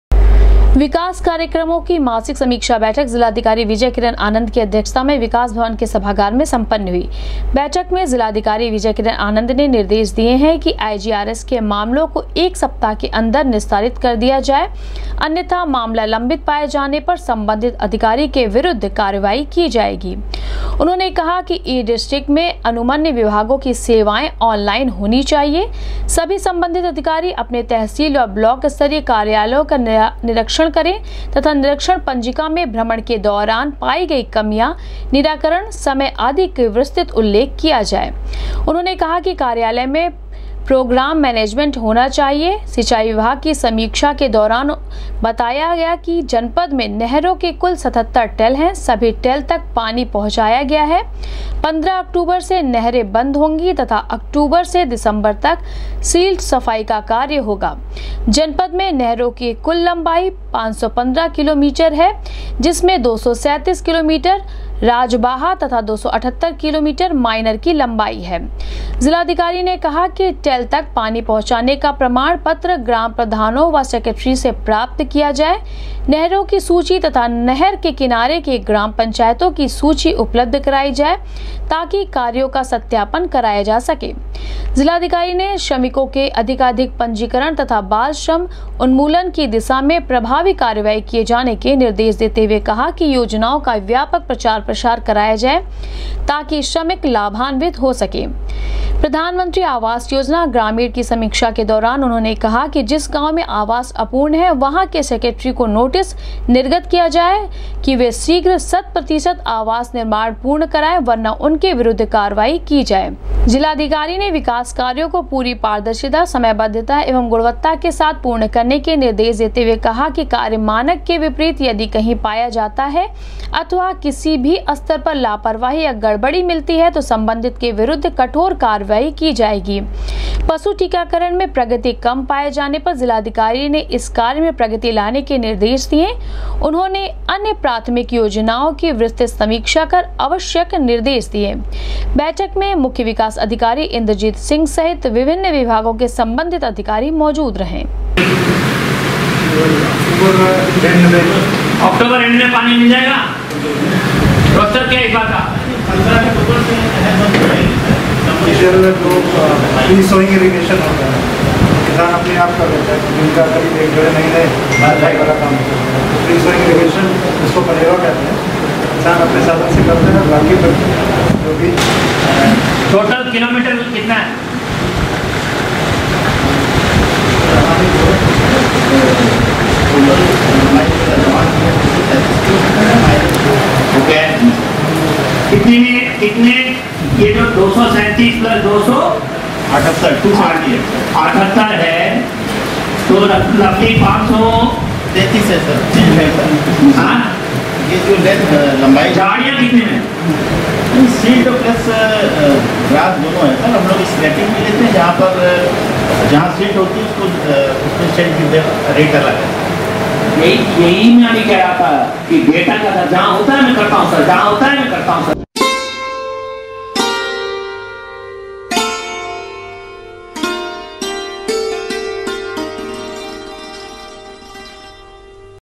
जिलाधिकारी की अध्यक्षता में सम्पन्न हुई विकास कार्यक्रमो की समीक्षा बैठक,आईजीआरएस के मामलों को एक
जिलाधिकारी की अध्यक्षता में सम्पन्न हुई विकास कार्यक्रमो की समीक्षा बैठक,आईजीआरएस के मामलों को एक सप्ताह के अन्दर निस्तारित करने के दिये निर्देश ,कहा,विकास कार्यो को गुणवत्तापूर्वक समय से करे पूरा